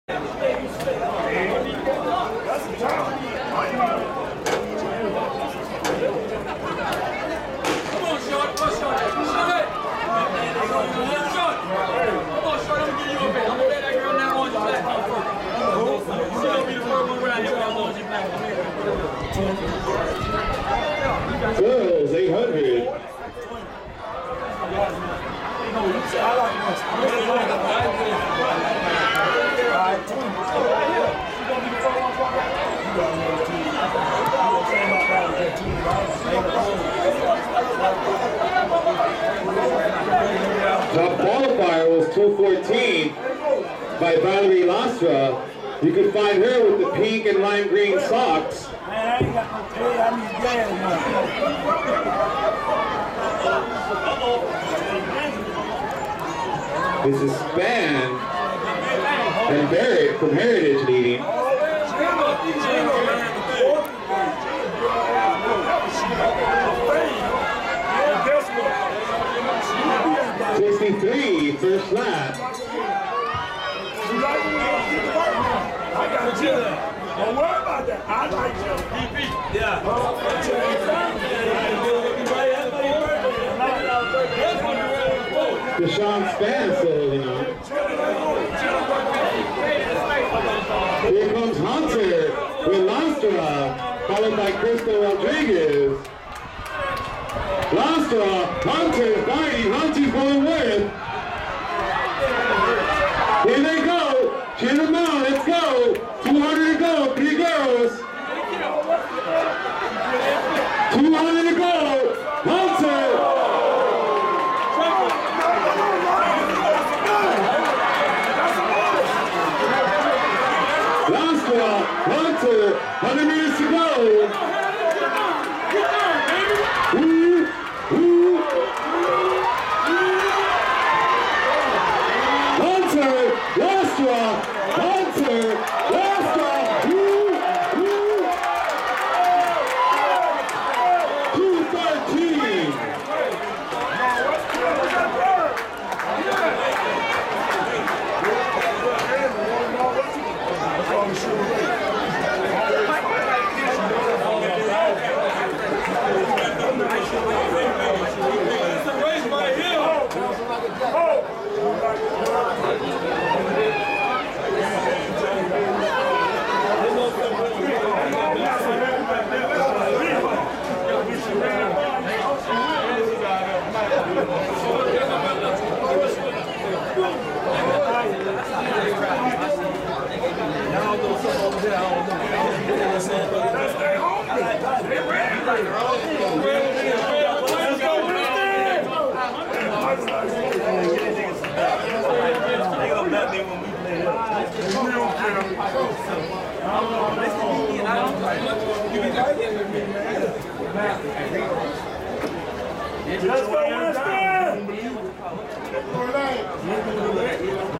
Girls 800 Meter Run Finals Louie Bing Memorial Classic - 2/16/2019
Finals
1 Valerie Lastra            11 Mater Academy          2:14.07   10
2 Le'nai Hunter             10 Miami Northwestern     2:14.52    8
3 Jadae Barrett             11 American Heritage      2:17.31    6
4 Krystal Rodriguez         10 Coral Reef             2:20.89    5
5 Noemi Kerjean             11 St. Brendan            2:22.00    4
6 Jaziah Ochoa              12 St. Thomas Aquinas     2:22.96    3
7 Rylee Pustilnik           11 Spanish River          2:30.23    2
-- Daishon Spann             12 Cocoa                      DNF